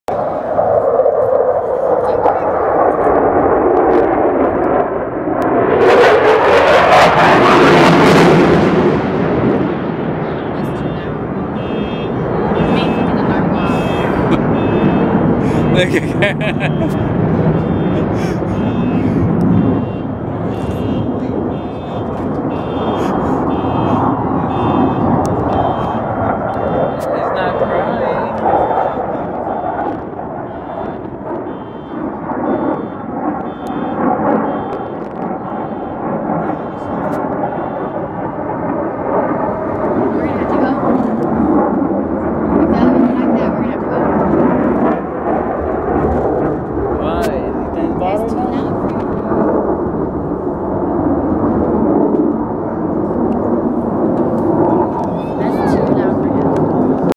Wings Over Houston 2009 Jets (1080p quality)
I think this is a F-15 but someone please correct me if I'm wrong.  This is a clip of a F-15 roaring through the skies at the 2009 Wings Over Houston event on October 31st.